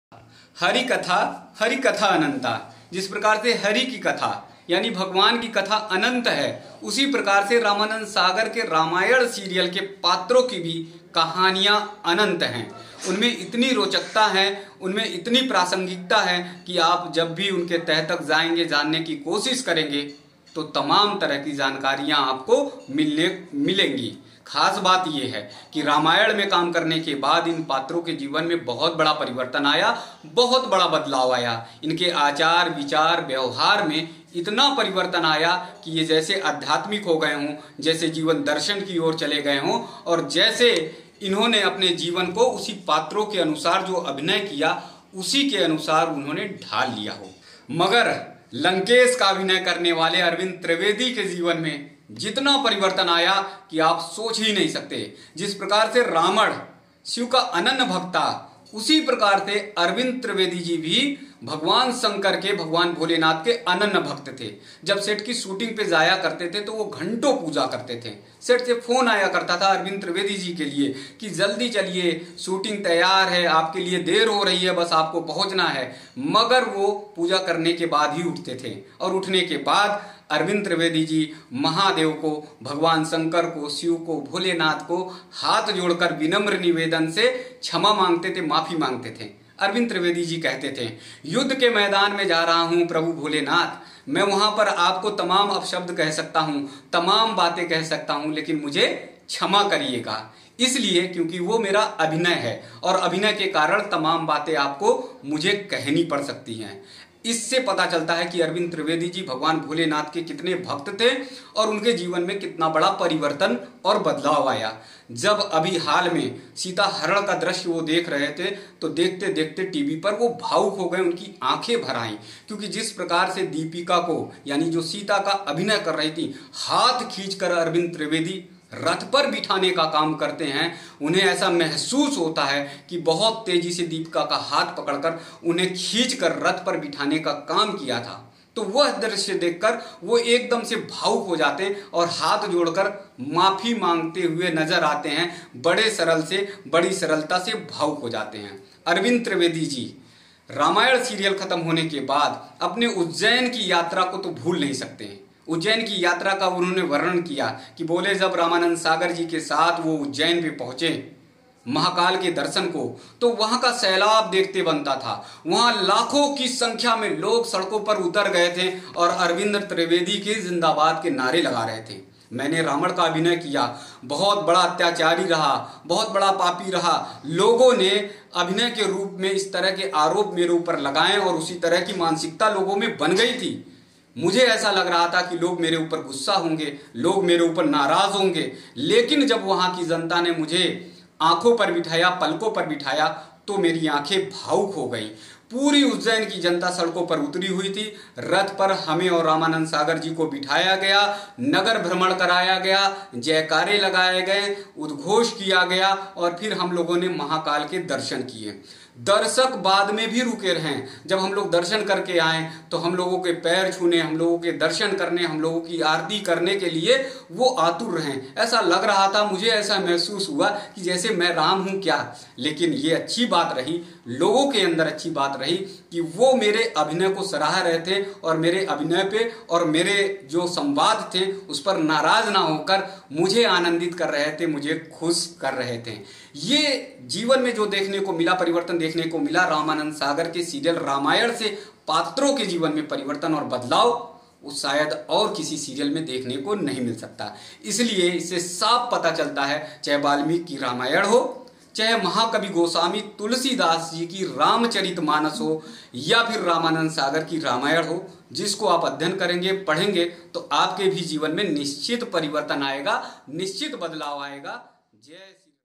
अरविंद त्रिवेदी (रावण) व्यक्तिगत जीवन में आज भी करते हैं इनकी पूजा।the news nonstop
अरविंद त्रिवेदी (रावण)व्यक्तिगत जीवन में किस के पुजारी  थे।

Ramayan#arvind travedi rawan#ramanand sagar#ujjen rath yattra#